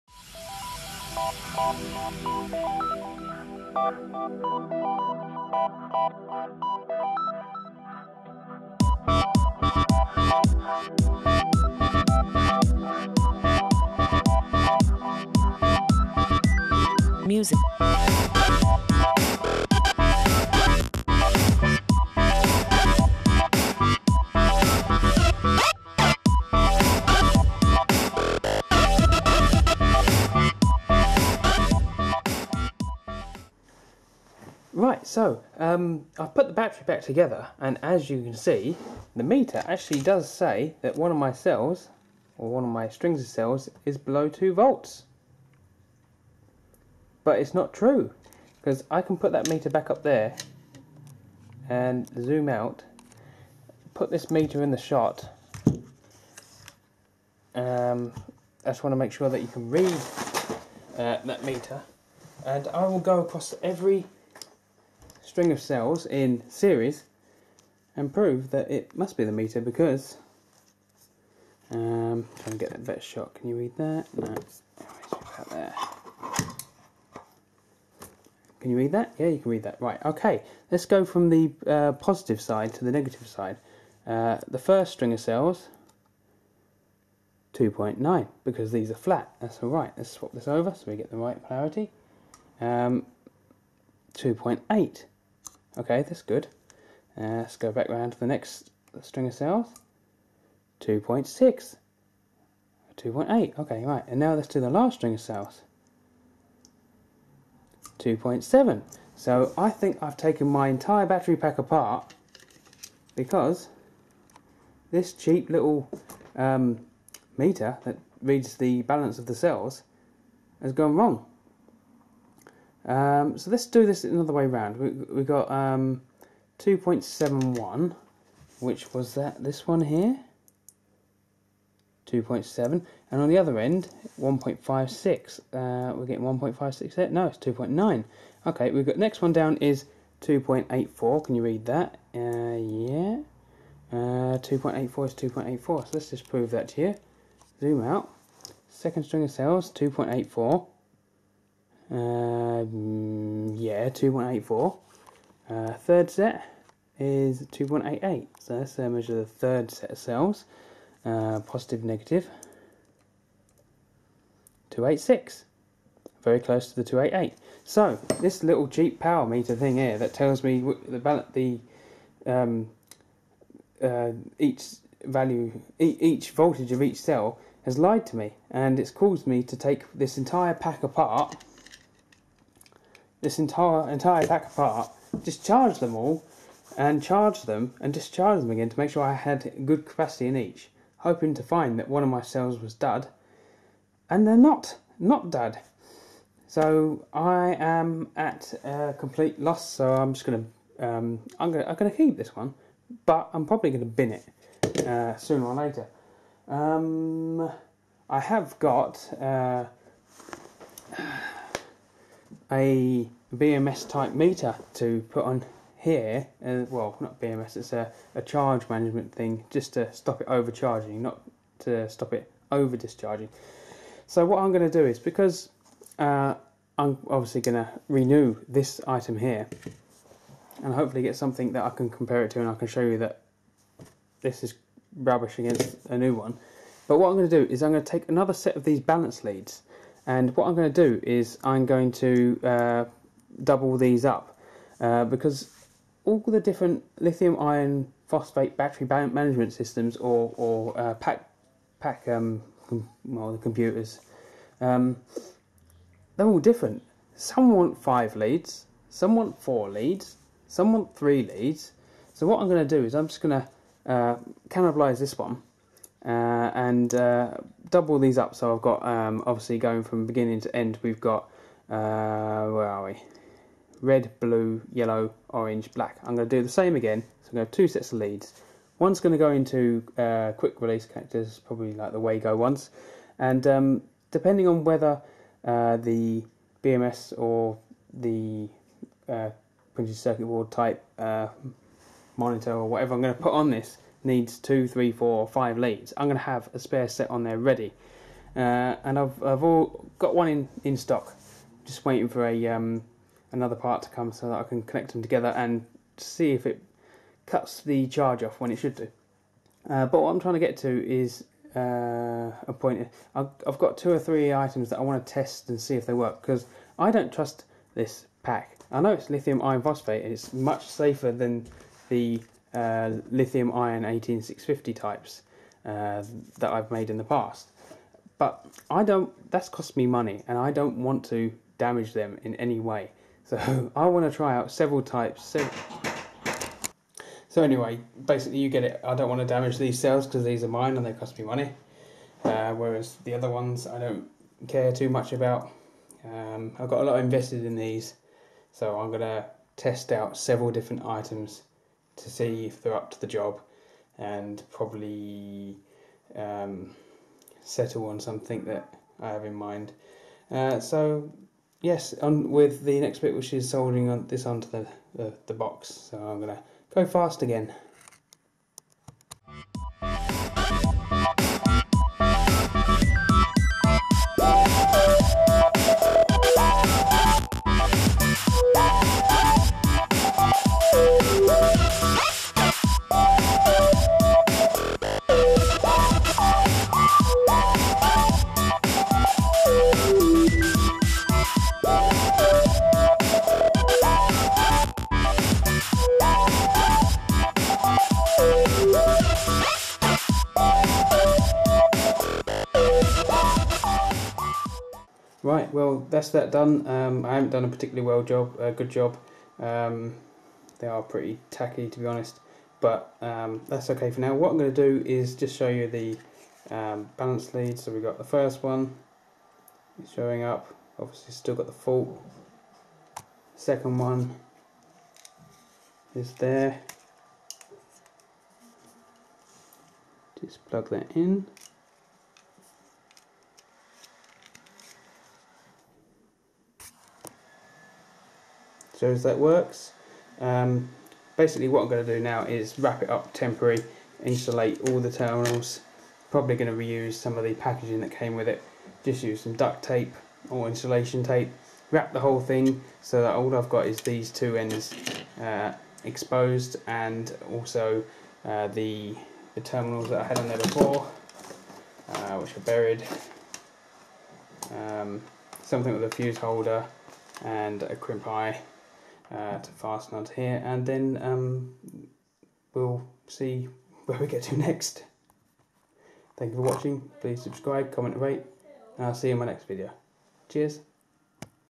lifepo4 part 6 found the fault at last
I finally figured out the fault! upon re-assembeling the pack and testing, I plugged in the cell moniter and the fault magically appeared! see for your self in the video!